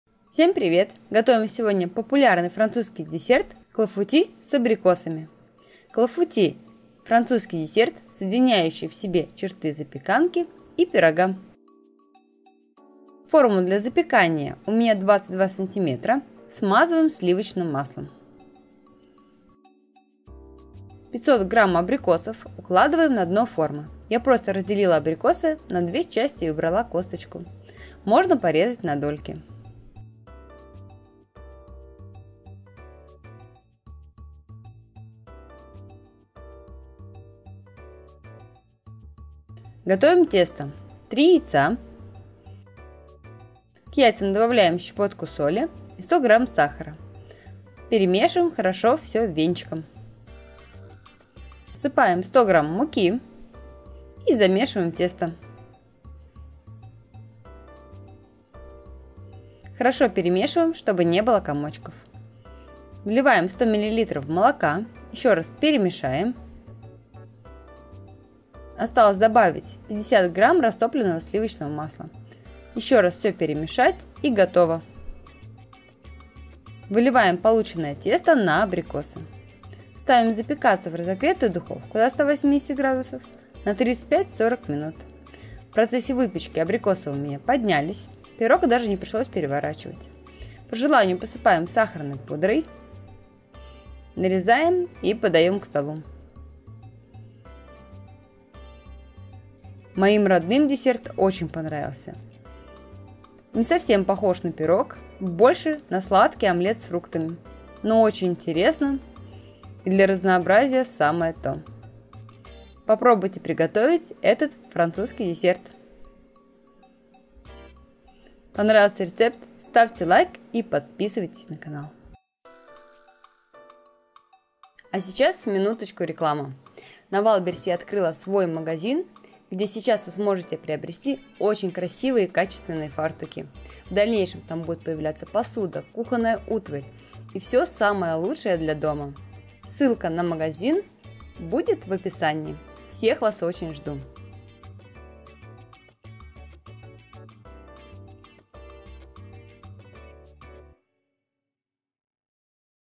Клафути с абрикосами - Популярный французский десерт у себя дома
Готовлю сегодня очень популярный французский десерт - Клафути с абрикосами. Интересный, необычный, сочный и вкусный. Это не классический пирог с абрикосами, но станет хорошей заменой.

Клафути с абрикосами:
Абрикосы - 500 г
Соль - щепотка

Клафути с абрикосами прекрасный десерт к чаю. Абрикосы можно менять на другие фрукты и ягоды.

А приобрести формы для выпечки, шоколад, какао и много всего интересного вы можете в кондитерском магазине BAKER Store

Для поддержания проекта: карта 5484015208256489 (сбербанк)